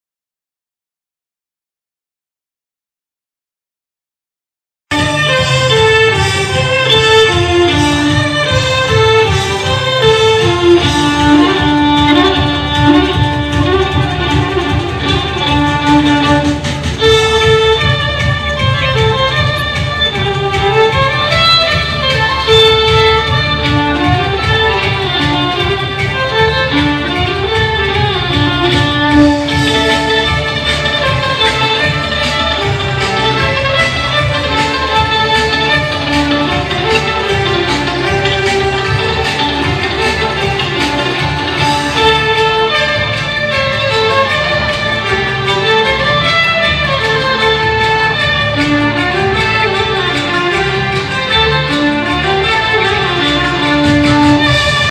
Absolute Pop Violin Ensemble: Electric Pop Violin (SaiMa).wmv
Electric Pop Violin perform by Jessica & Kelly in Tai Kuk Wah Si Temple.